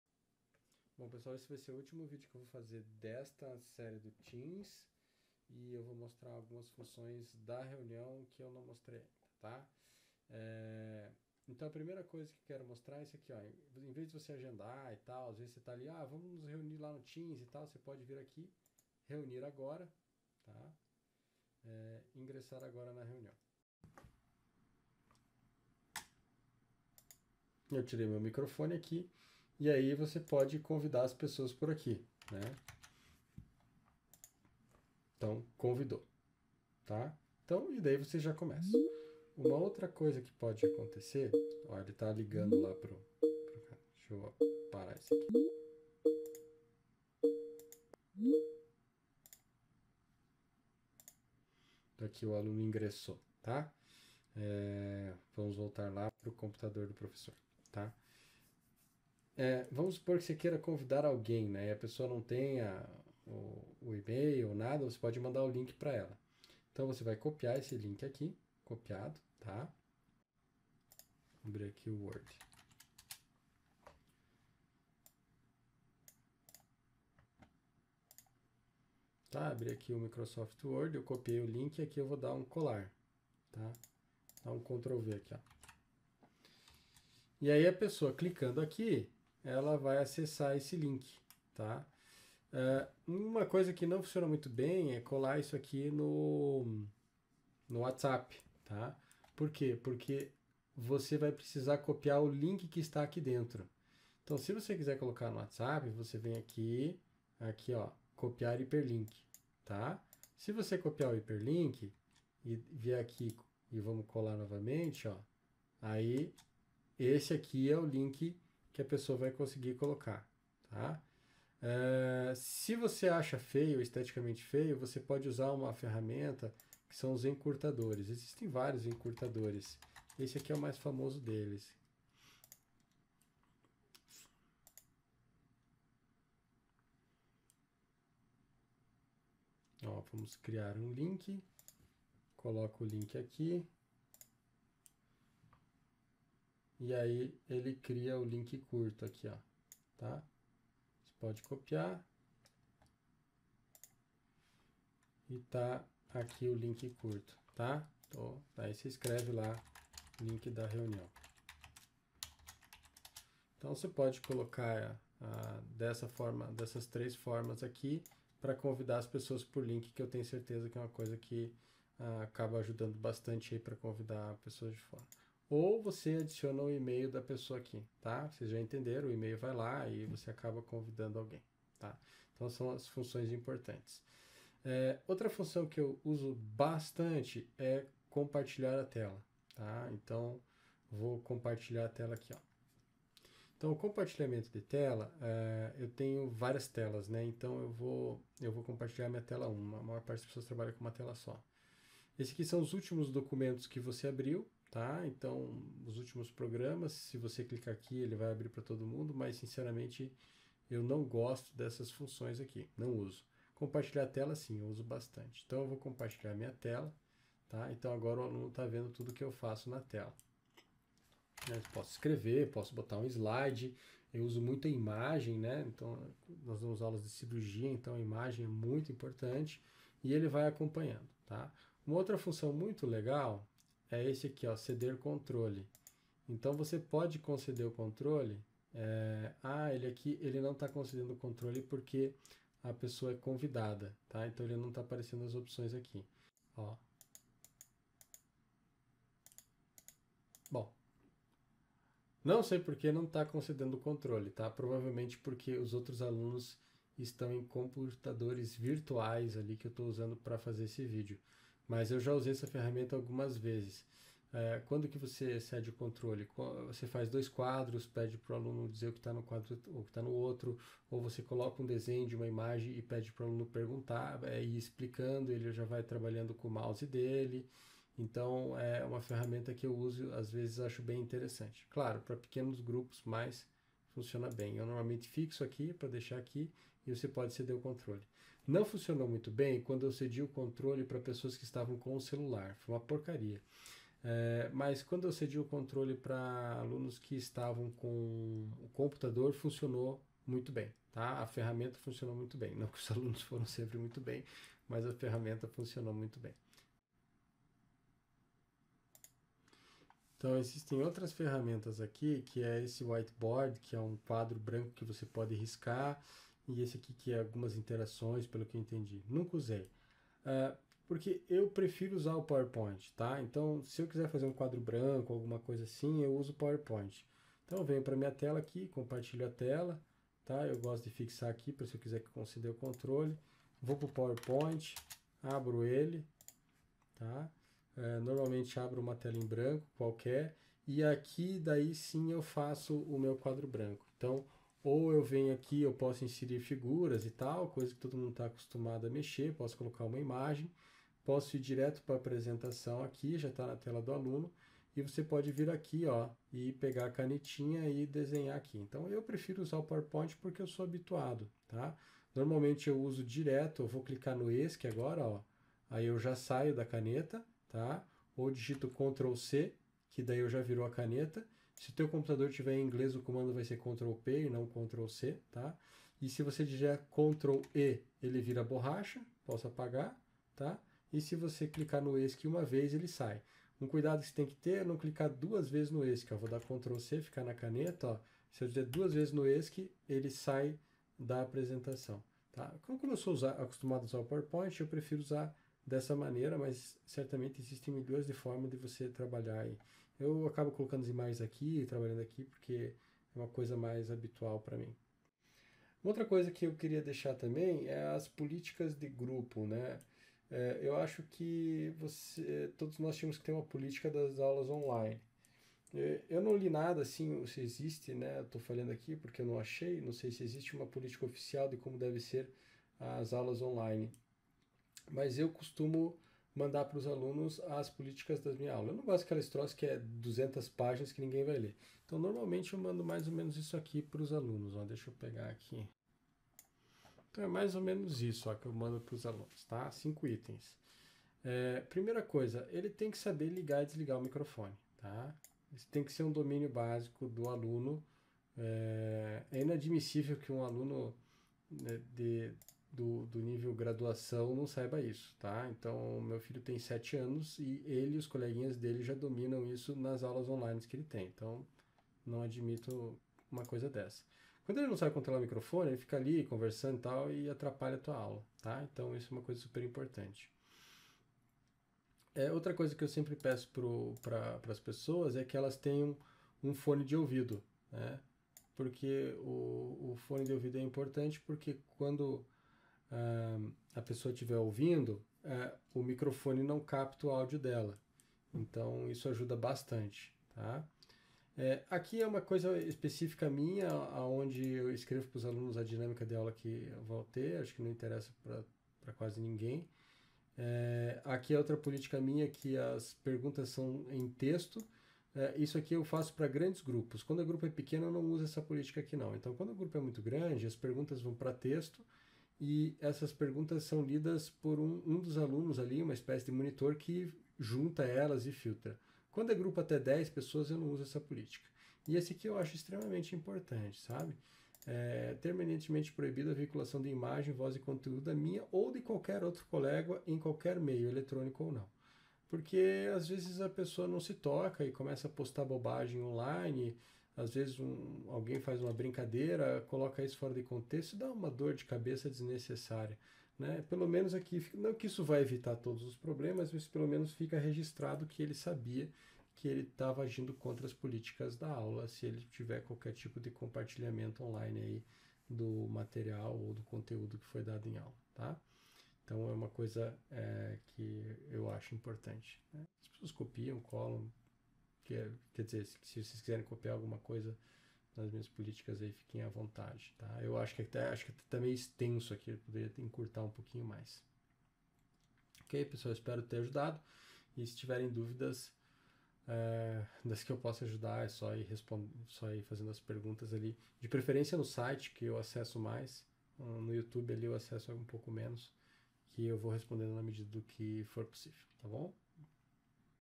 Microsoft temas – parte 6 – compartilhamento de tela, convite por link público e políticas
Microsoft temas – parte 6 – compartilhamento de tela, convite por link público, reuniões imediatas e políticas da aula.